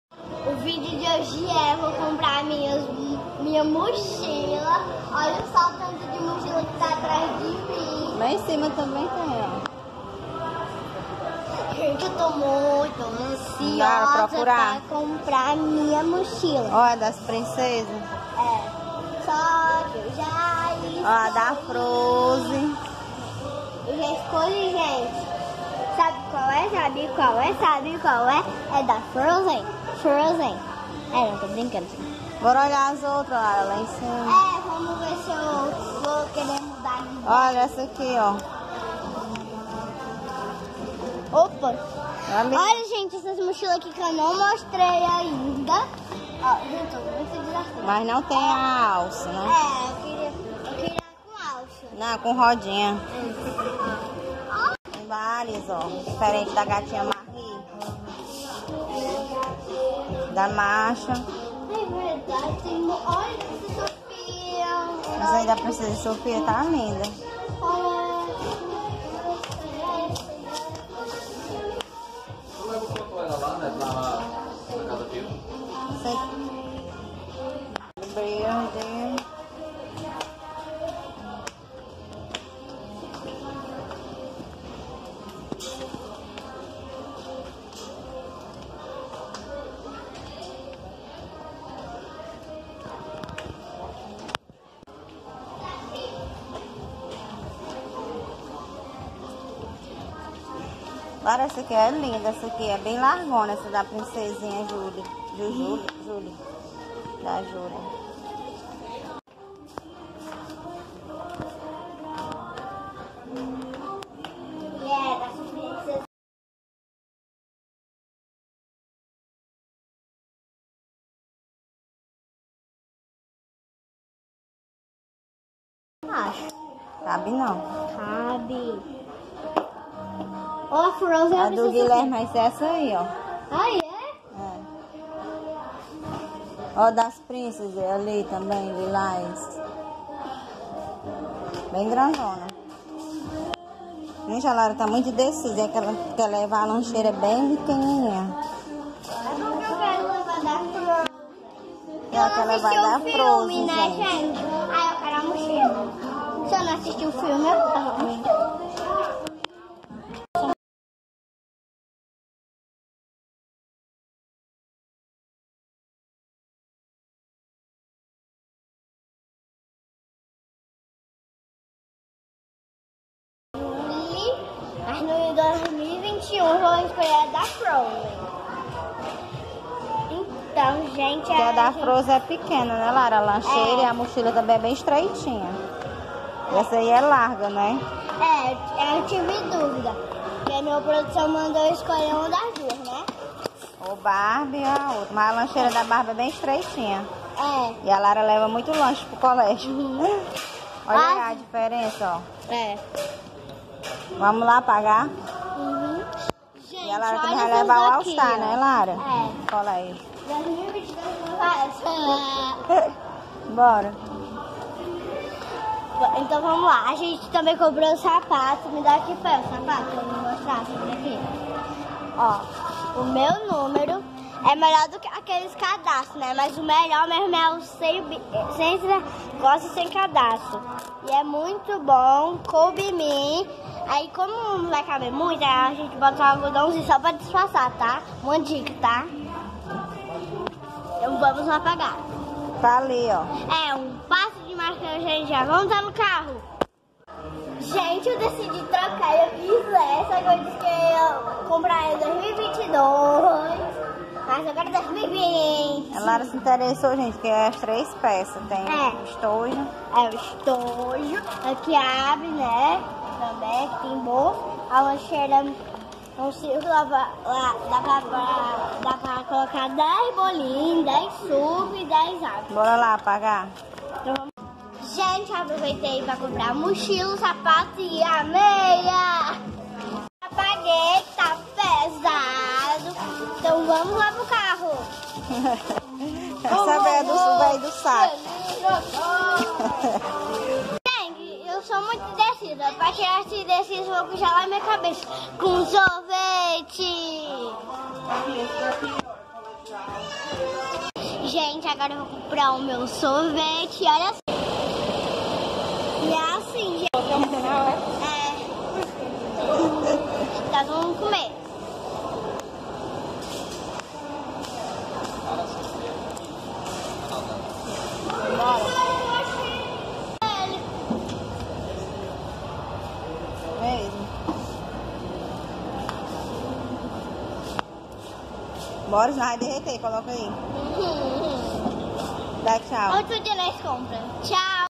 Mochila escolar 2020
Oi meus amigos e amigas, tudo bom com vocês? O vídeo de hoje é comprando minha mochila escolar desse ano de 2020, ela é uma fofura. Gostou do vídeo?

QUER MANDAR CARTINHAS E PRESENTINHOS:
Lara Sophia
Rua planaltina N° 1751 – Planalto Ayrton Senna
Cep: 60760480
Fortaleza-Ce